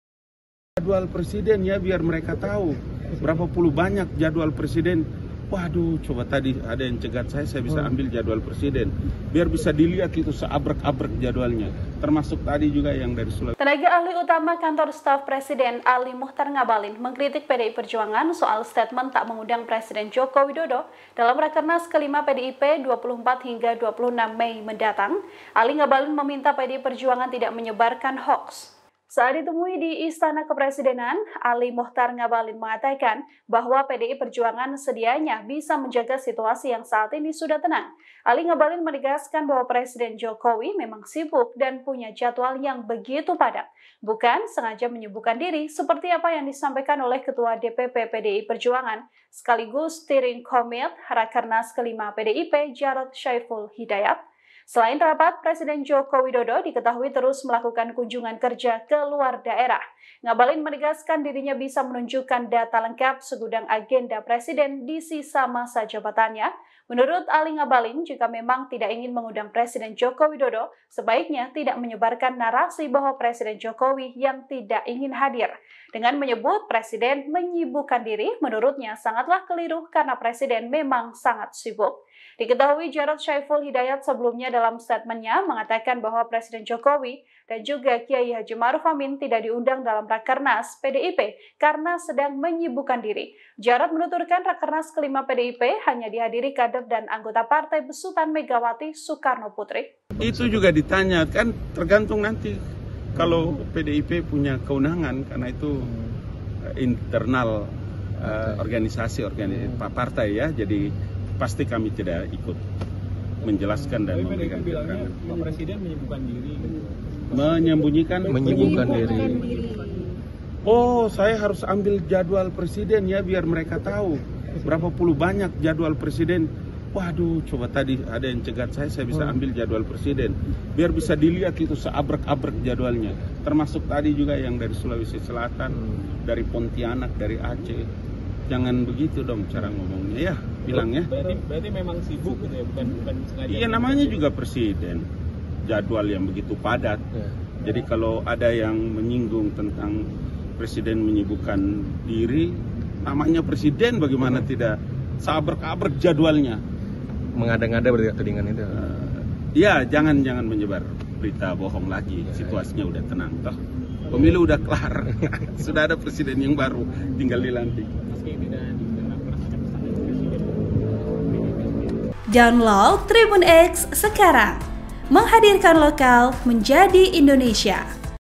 PDIP Tuduh Jokowi Sibukkan Diri, Ngabalin Pasang Badan: Jangan Sebar Berita Bohong Lagi!
TRIBUN-TIMUR.COM -

(TRIBUN-TIMUR.COM)